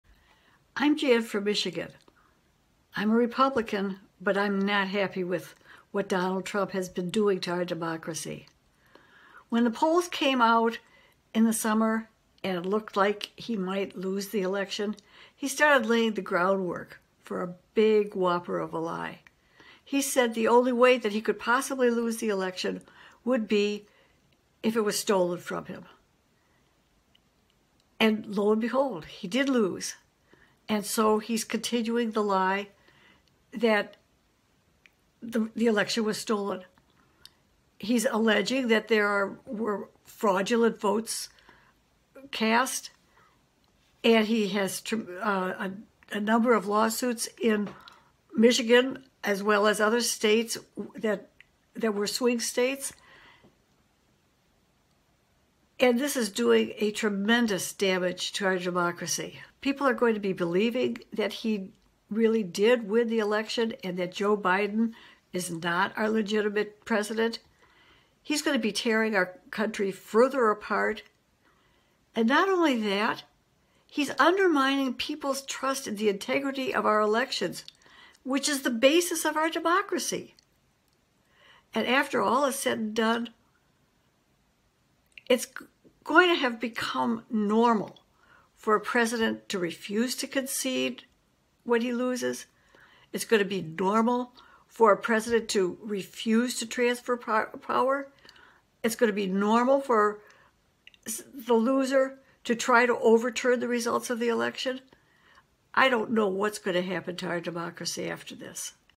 We can't let Trump's behavior become normal
"He's undermining people's trust and the integrity of our elections, which is the basis of our democracy. And after all is said and done, it's going to have become normal for a president to refuse to concede when he loses."